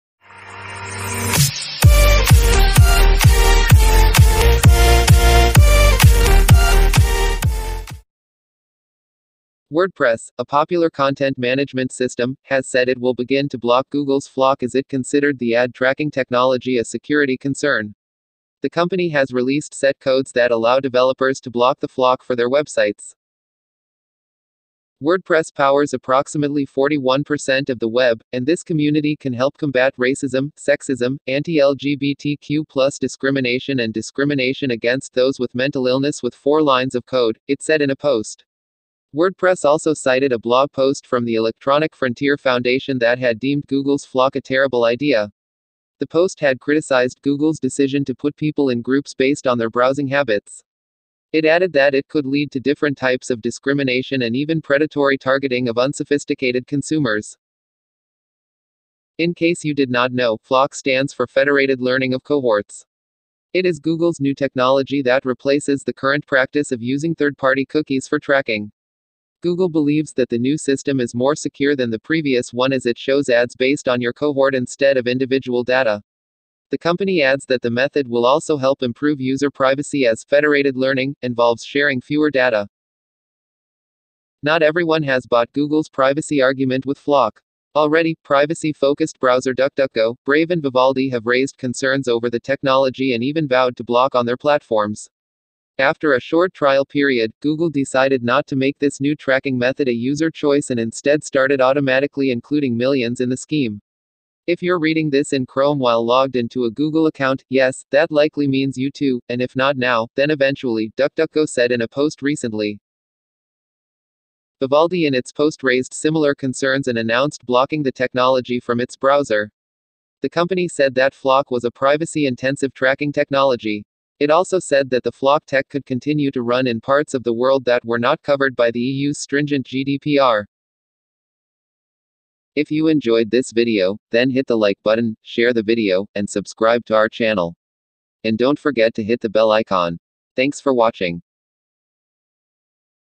Google’s new ad tracking tech to be blocked by WordPress
WordPress, an open-source content management system, said it will block Google’s new ad tracking technology as the company sees it as a security concern.

Google’s Federated Learning of Cohorts (FLoC) is a new ad tracking feature that will replace third-party cookies. Google started testing the feature recently; it uses an algorithm to place people with similar browsing histories in a group for target advertising.

Google has not rolled out the feature completely yet, however, a number of browsers including DuckDuckGo, Brave, Vivaldi and Mozilla have opted out of FLoC.

WordPress, that powers about 41% of the web, has released four lines of code that can help developers block FLoC for their websites.

For users who want to opt into FLoC, they will be able to do so themselves. Besides, with few more lines of code, WordPress versions that support privacy settings can have an on-off toggle to enable websites to opt in for Floc.

“While it is indeed unusual to treat a new “feature” this way, there is precedent in that something that was not strictly a security vulnerability in comments was back-ported to previous versions for the good of the community as a whole,” WordPress said in a blog post.

It also mentioned a post by the Electronic Frontier Foundation, that Google’s FLoC a terrible idea. It further said that placing people in groups based on their browsing habits is likely to facilitate employment, housing and other types of discrimination, as well as predatory targeting of unsophisticated consumers.